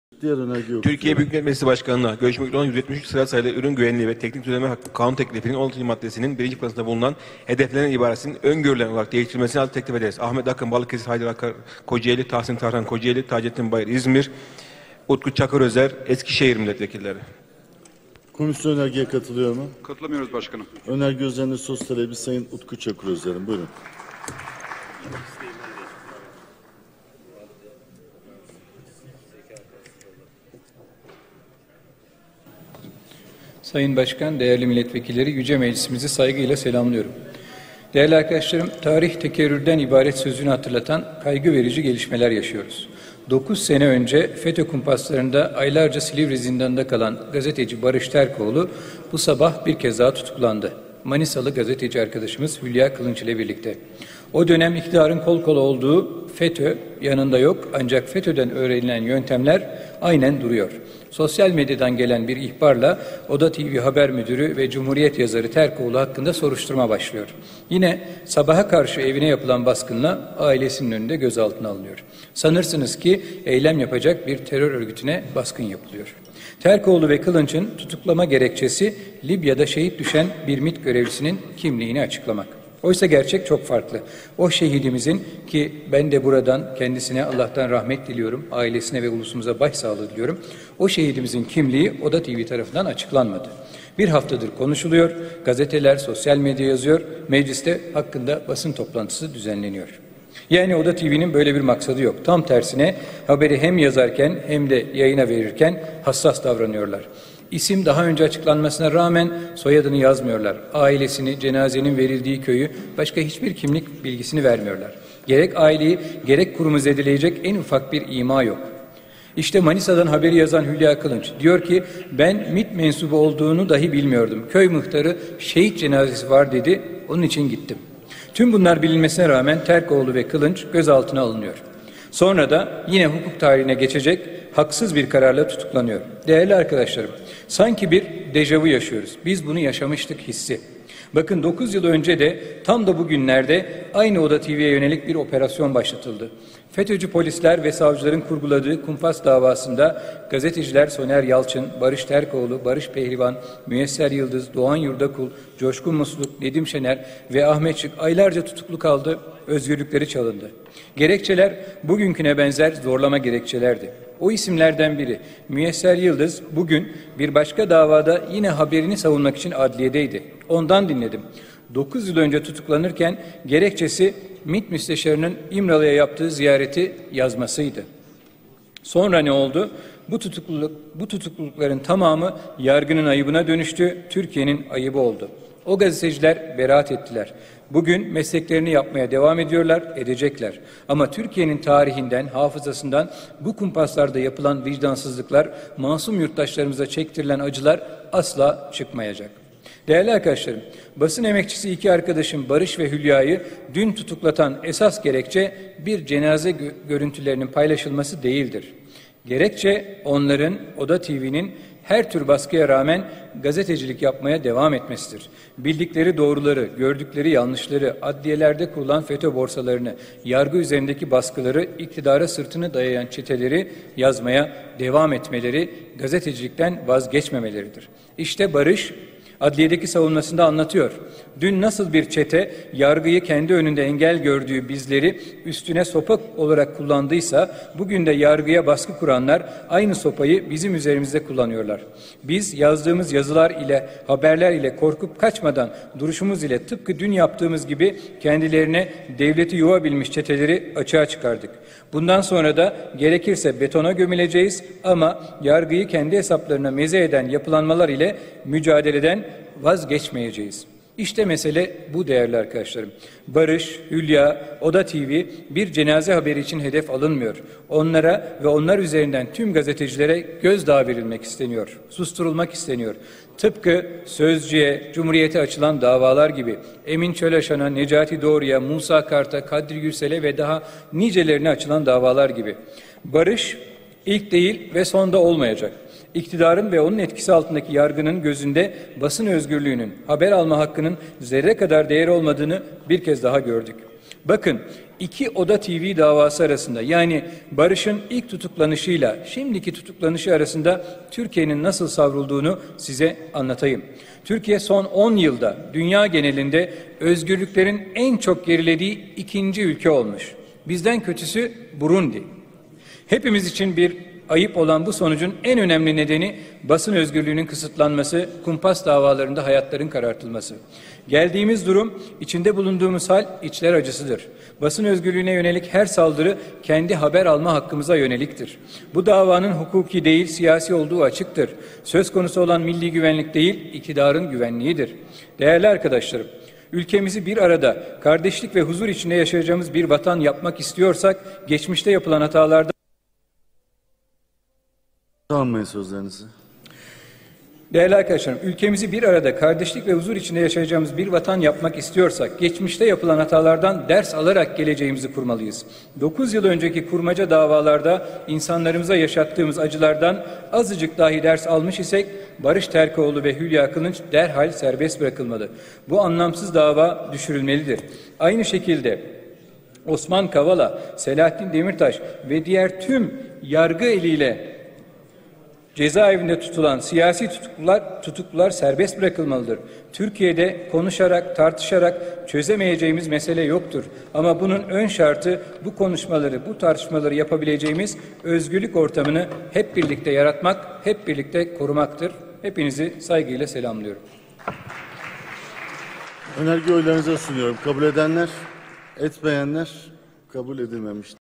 CHP ESKİŞEHİR MİLLETVEKİLİ UTKU ÇAKIRÖZER MECLİS KONUŞMASI-16.MADDE-5 MART 2020
ÜRÜN GÜVENLİĞİ VE TEKNİK DÜZENLEMELER KANUN TEKLİFİ
KARMA İÇERİKLİ WEB SİTESİ: